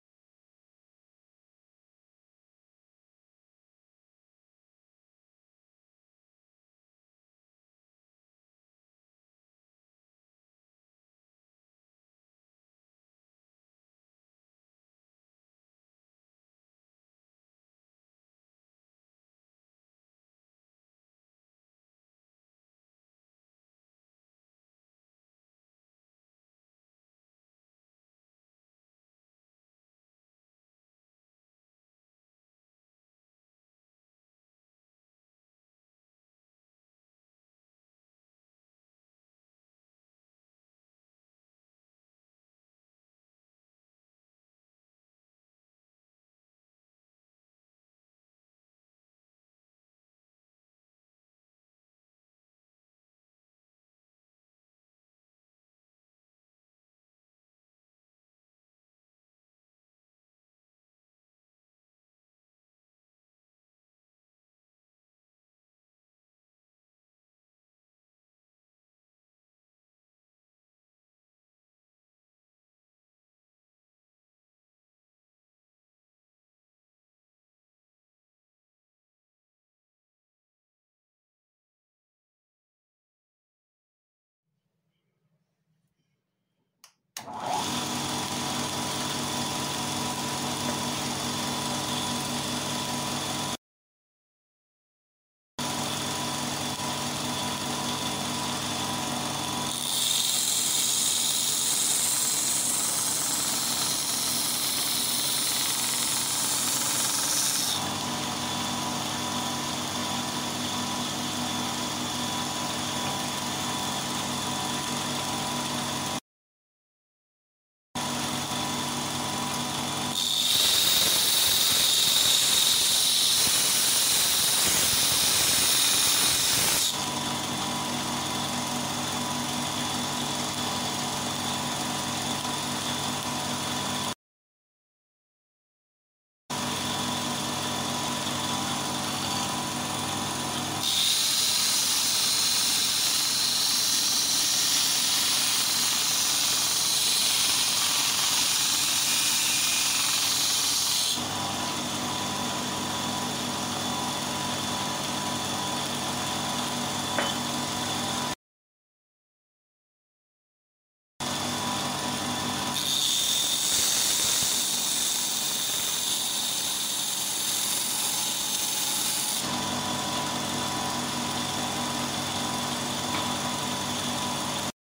Компактный гриндер.
Компактный гриндер, вес 9кг, мотор 370 Ватт, 3000 об/мин, под ленту 610х50